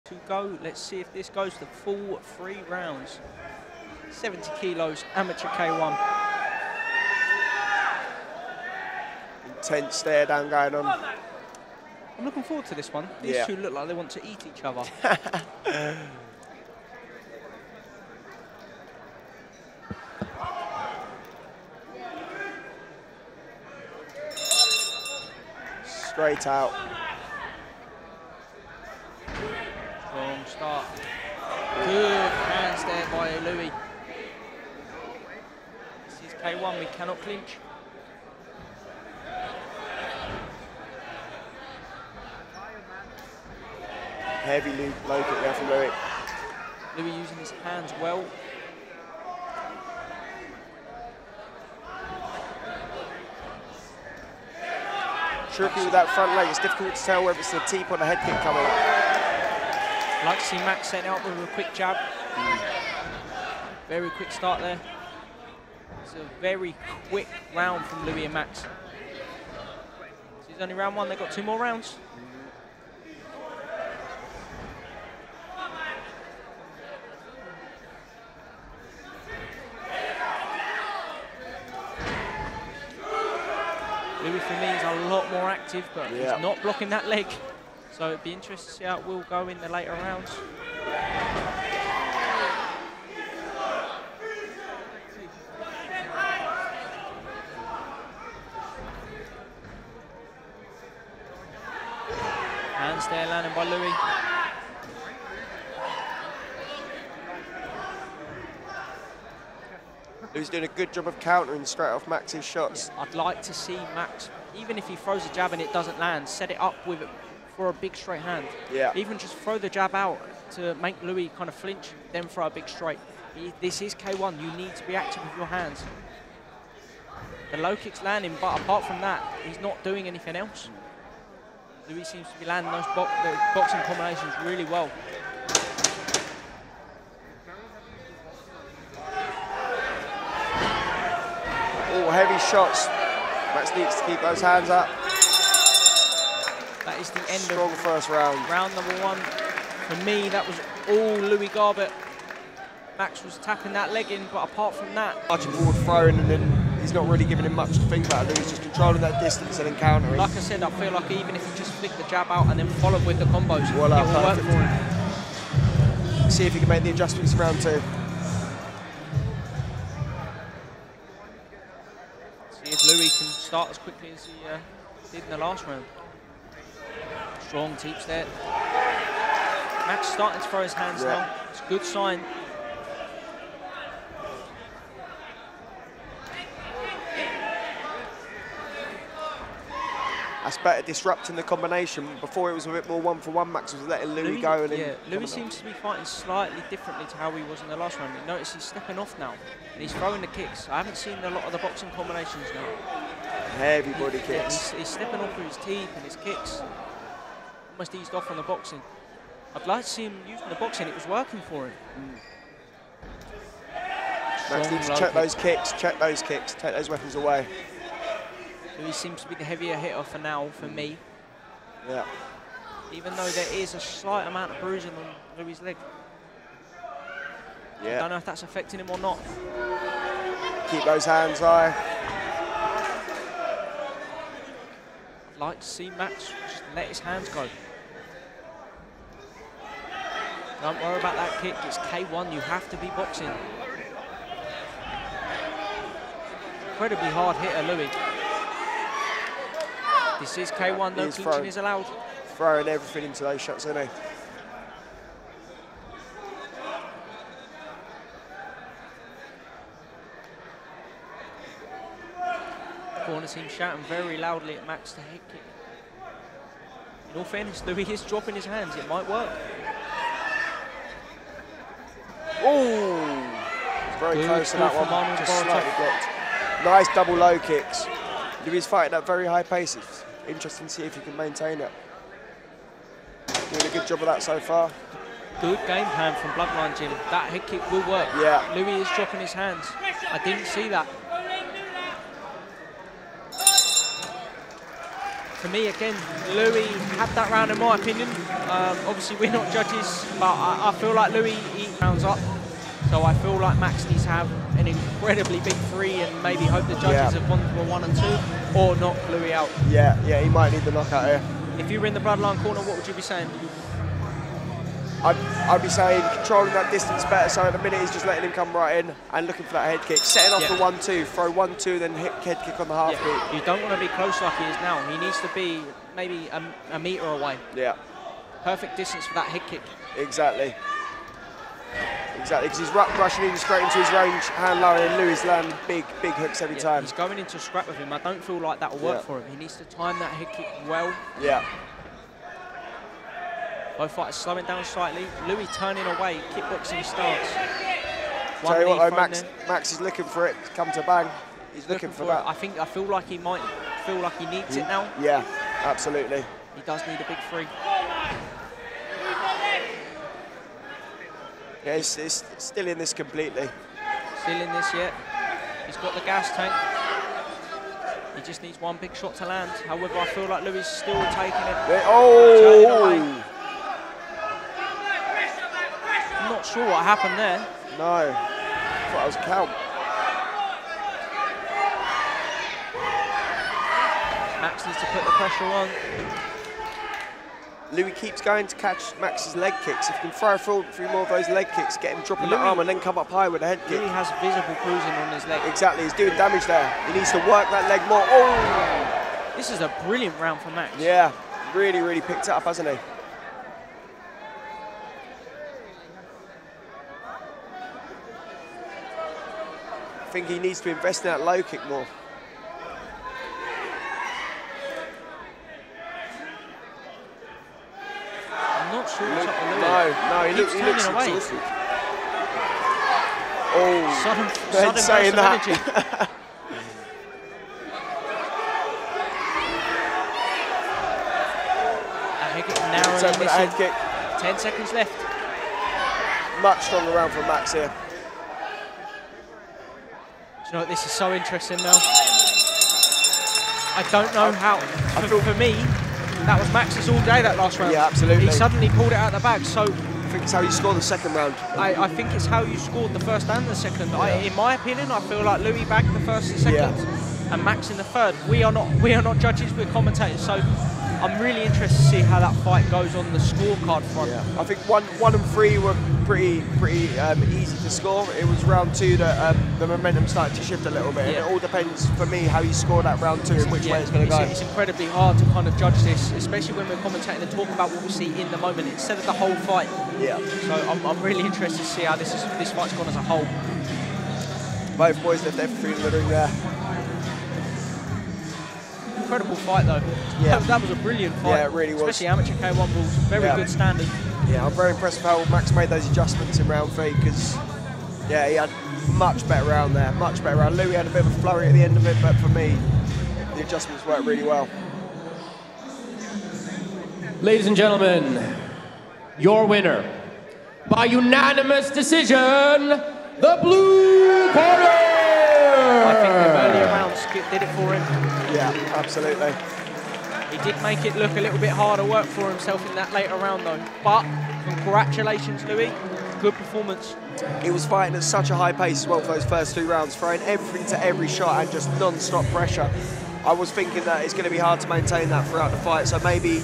Max Ciesielski Vs Louie Garbett 70kg Amateur K1 bout
Another amazing fight filled to the brim with action.

An Amateur K1 bout that left us on the edge of our seat trying to get a better look at all the fast paced action.

Both fighters did great and we hope to say way more of them.

What do you think about this fight?

This K1 fight was live streamed on Leapfrog Fight TV